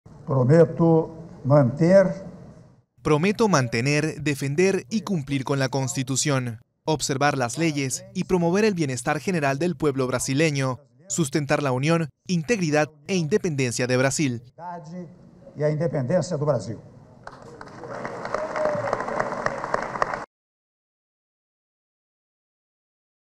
URGENTE: Michel Temer asumió la presidencia de Brasil
Michel Temer juró el miércoles como nuevo presidente de Brasil poco después de la destitución de Dilma Rousseff por parte del Senado.